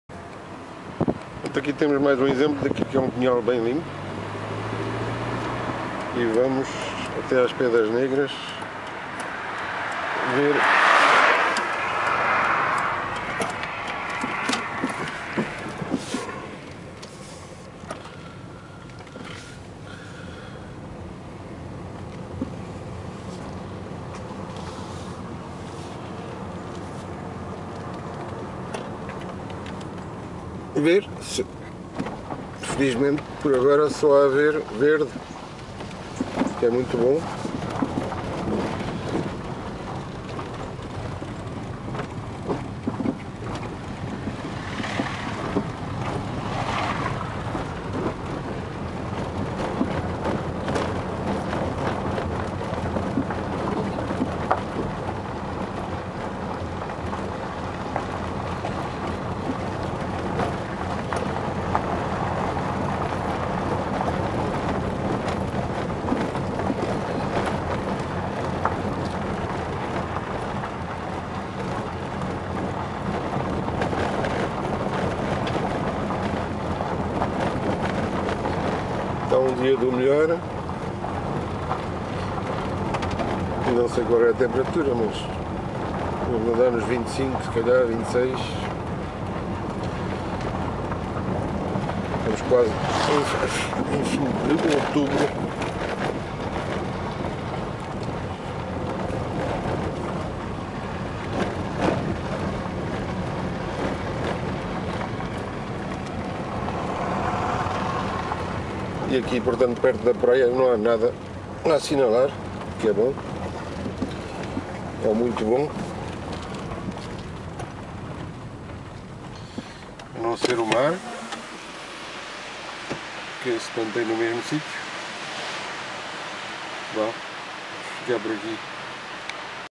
Praia das PEDRAS NEGRAS visitada a 23 OUTUBRO , APÓS O DIA 15 de Outubro 2017
Visita a PEDRAS NEGRAS / SAMOUCO , após o incêndio de 15 de Outubro 2017 , TUDO OK por aqui  , não é caso do outro lado da estrada Atlântica -  S. Pedro de Moel , Marinha Grande , Leiria , Pinhal do Rei , Mata Nacional , Pinhal de Leiria  , Estrada Atlântica , Portugal .